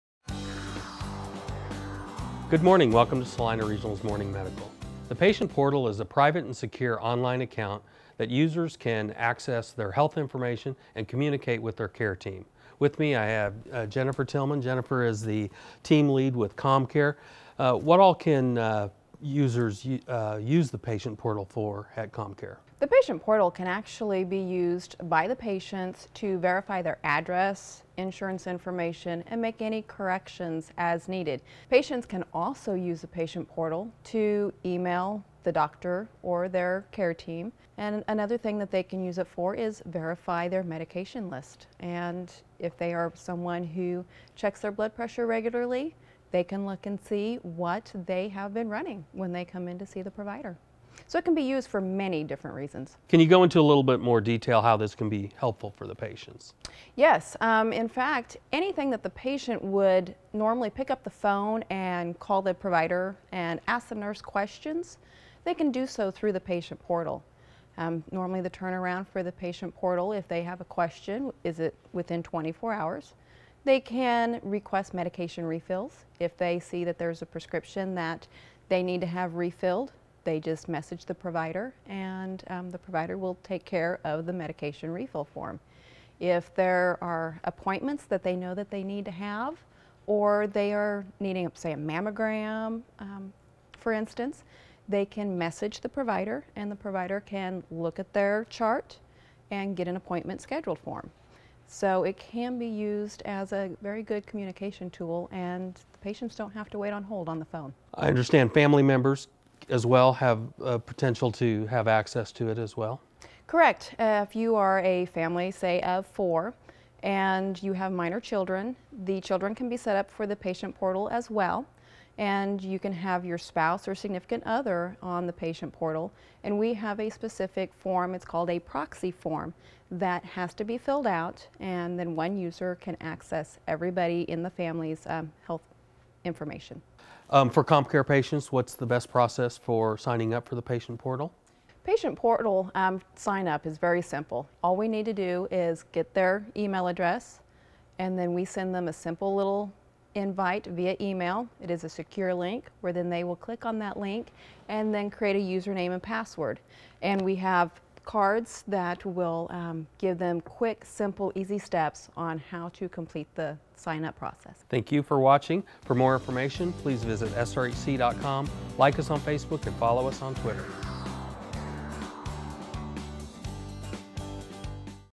Morning Medical | COMCARE patients use valuable online resource
COMCARE patients are signing up for convenience and peace of mind when they open a Patient Portal account. The Patient Portal is an online access point where patients can safely and privately view their health history as well as communicate with their doctor. Jennifer Tillman, Team Lead at COMCARE on Santa Fe street, provides details on the benefits of having an account.